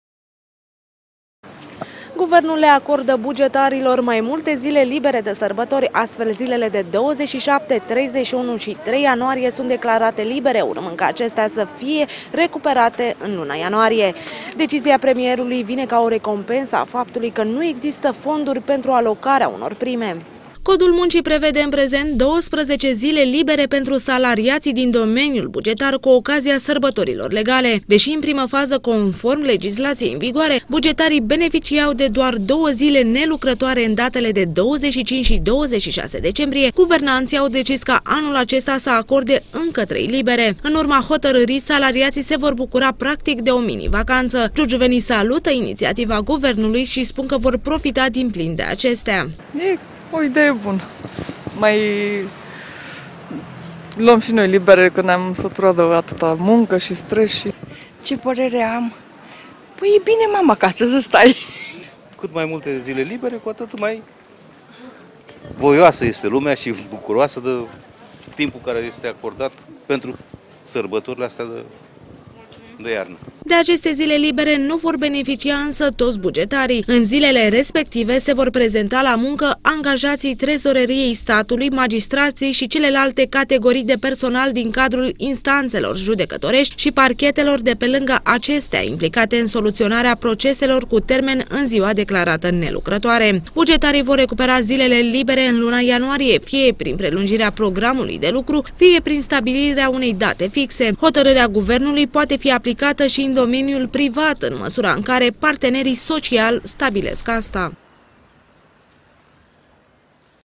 MINI-VACANTA DE SARBATORI
Bugetarii vor avea parte de o mini-vacanță de sărbători. Guvernul a decis ca zilele de 27, 31 decembrie și 3 ianuarie să fie nelucrătoare. Astfel, salariații români se vor bucura de numai puțin de 7 zile libere, în perioada Sărbătorilor de iarnă.  Practica vacanțelor prelungite este o inițiativă a Guvernului Ponta și a început încă de anul trecut.